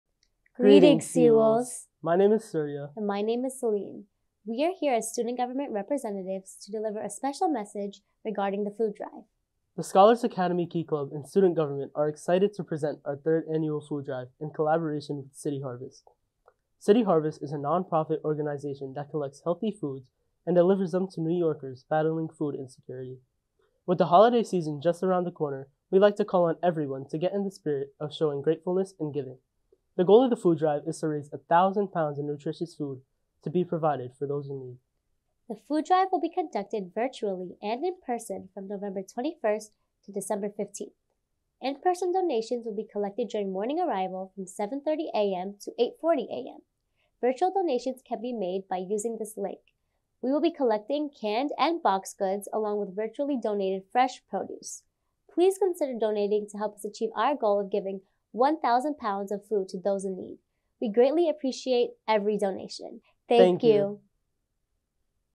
Food Drive | A Scholars' Academy Announcement
The Scholars’ Academy Key Club and Student Government are excited to present our 3rd Annual Food Drive in collaboration with City Harvest. City Harvest is a non-profit organization that collects healthy foods and delivers them to New Yorkers battling food insecurity. With the holiday season just around the corner, we’d like to call on everyone to get in the spirit of showing gratefulness and giving. The goal of the Food Drive is to raise 1,000 pounds of nutritious food to be provided for those in need.
The Food Drive will be conducted virtually and in-person from November 21st to December 15th. In-person donations will be collected during morning arrival from 7:30 AM to 8:40 AM. Virtual donations can be made by using this We will be collecting canned/boxed goods along with virtually donated fresh produce. Please consider donating to help us achieve our goal of giving 1,000 pounds of food to those in need.